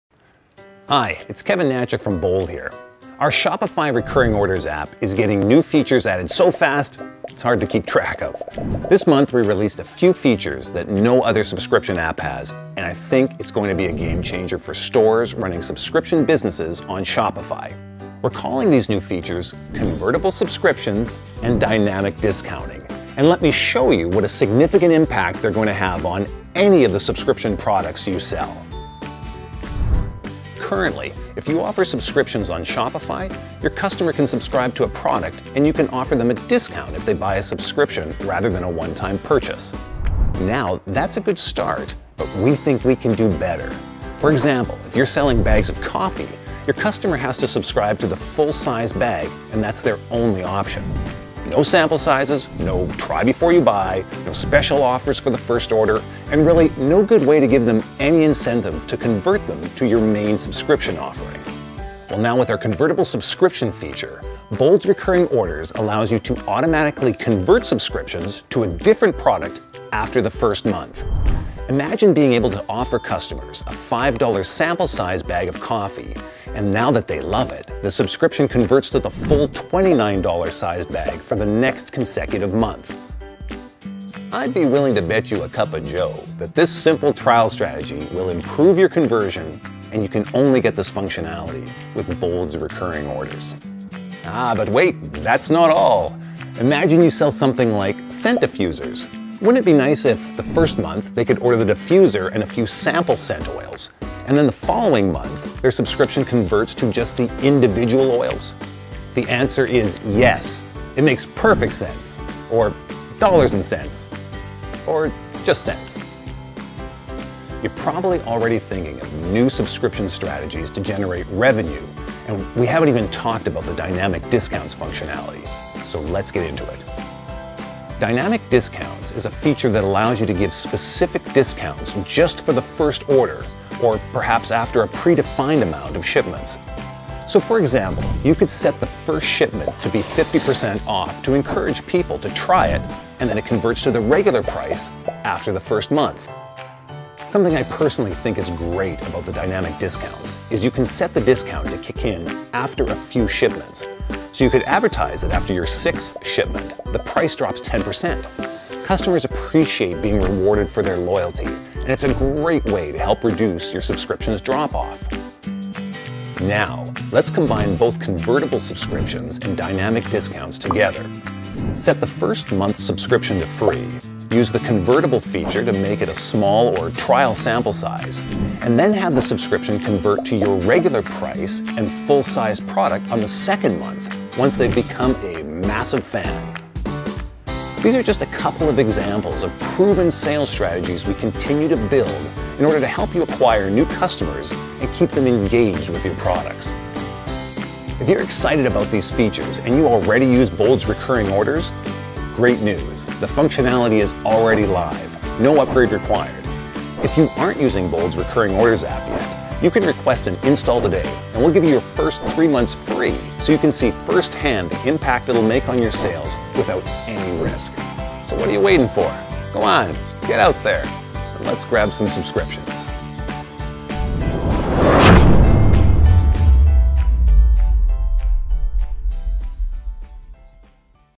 Introducing Convertible Subscriptions & Dynamic Discounts for Subscriptions on Shopify.
Today we announce what we believe will change the game for most subscription, and subscription box companies on Shopify. It's called Convertible Subscriptions and Dynamic Discounts, and our beta users are already doubling and tripling conversions!

The biggest challenge when selling subscriptions on Shopify is getting people to commit to buying a subscription, and if they buy, getting them to keep it month after month. The two features released today drastically help with both those problems